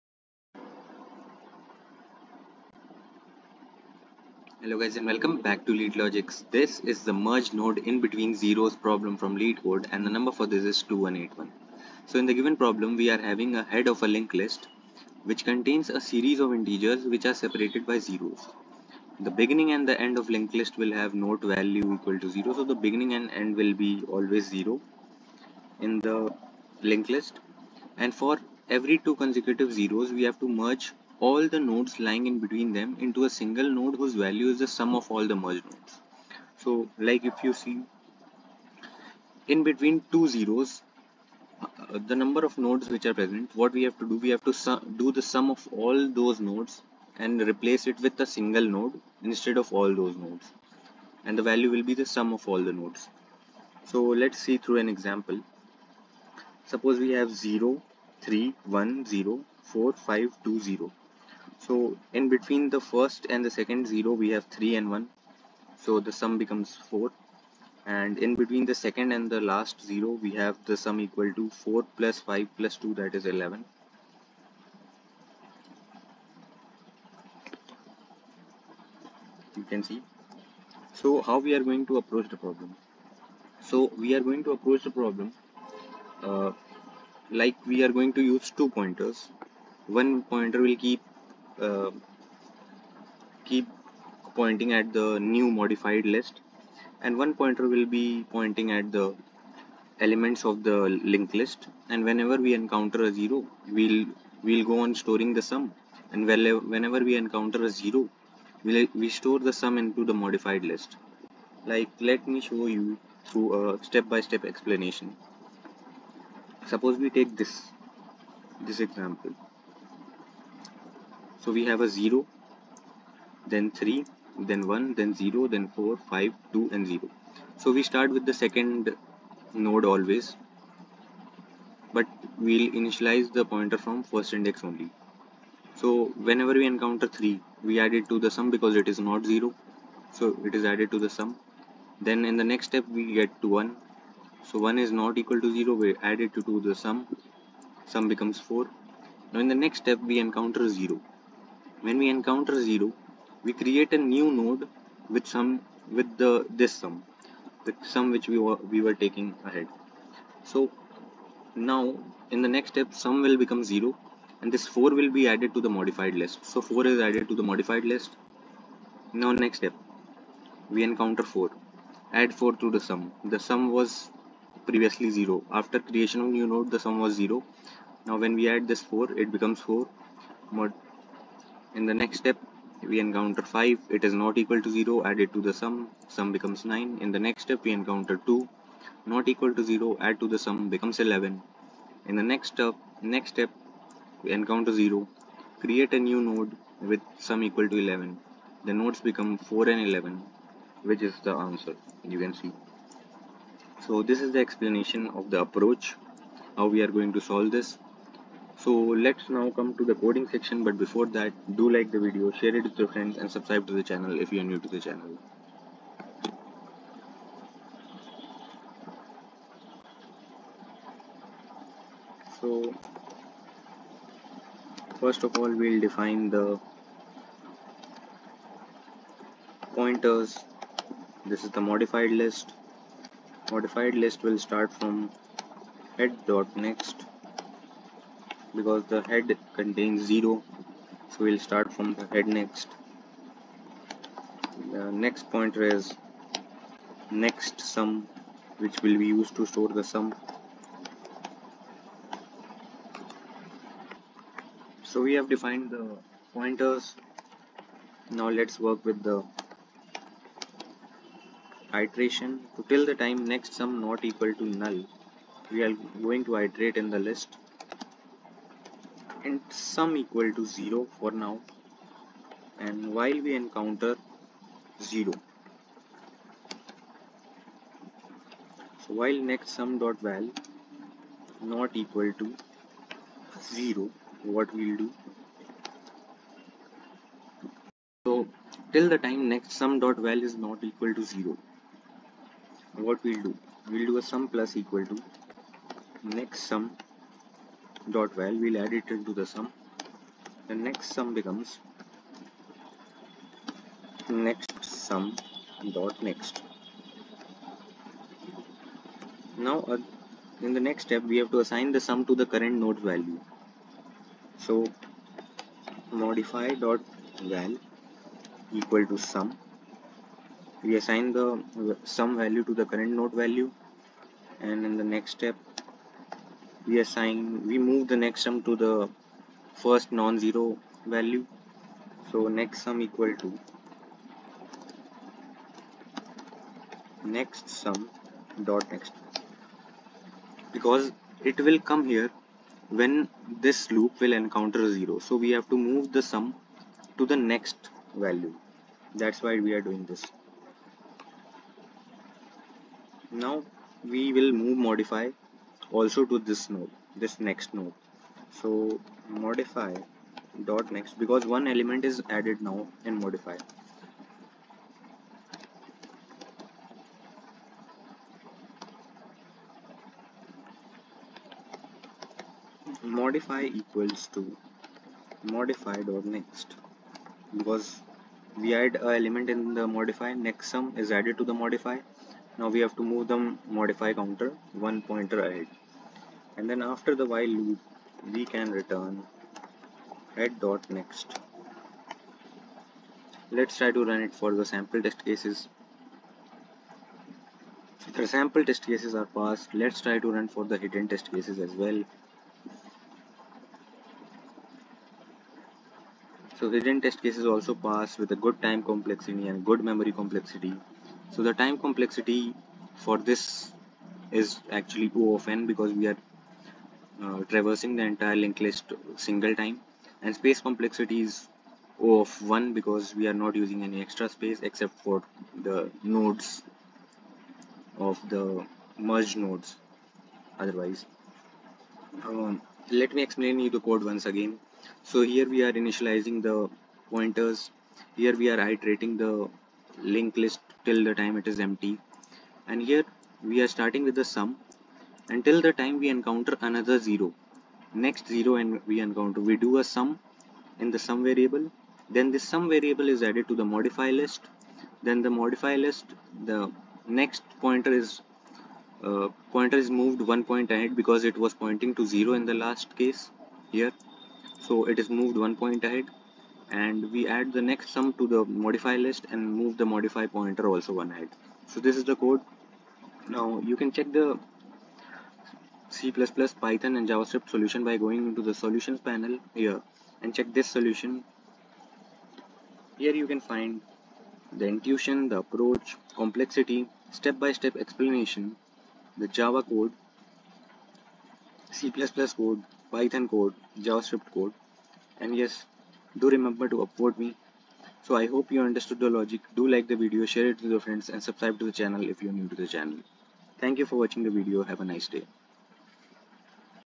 ✅ Merge Nodes in Between Zeros - LeetCode 2181 - Single Pass - O(N) - Explained in Detail
In today's video, we're tackling an interesting linked list problem where we need to merge nodes between zeroes. Given a linked list containing integers separated by zeroes, we aim to sum the nodes between every two consecutive zeroes and return a modified list without any zeroes.

We'll walk through a step-by-step explanation using tables to visualize the process, making it easy to follow and understand. Whether you're a beginner or an advanced programmer, this video is perfect for enhancing your coding skills and understanding linked list manipulations.

Example 1:

Input: [0, 3, 1, 0, 4, 5, 2, 0]
Example 2:

Input: [0, 1, 0, 3, 0, 2, 2, 0]
Output: [1, 3, 4]

Code Implementations:

C++
Python
JavaScript